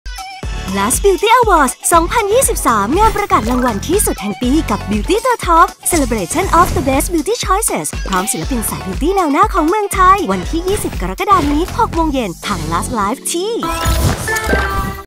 LazBEAUTYAwards2023 งานประกาศรางวัลที่สุดแห่งปีกับบิวตี้ตัวท็อป
ลุ้นไปกับ LazBEAUTYAwards2023 งานประกาศรางวัลที่สุดแห่งปีกับบิวตี้ตัวท็อป! จัดใหญ่เอาให้ใจเต้นระรัว 💕
น้องลาซการันตีความปังแบบตัวแม่ รู้แน่ชิ้นไหนต้องมี ที่นี่ที่เดียว! รับรองเซอร์ไพรส์เพียบ ✨

📍 20 ก.ค. | ไปรษณีย์กลาง บางรัก
👉 17.00 น. สามารถร่วมชม BEAUTY CARPET
📱 18.00 น. เป็นต้นไป ดูไลฟ์ที่

แจก Code พิเศษ!
แค่กรอก LazBEAUTYAwards ลดเลย 100 บาท (ใช้ได้วันที่ 21-24 ก.ค. เท่านั้น)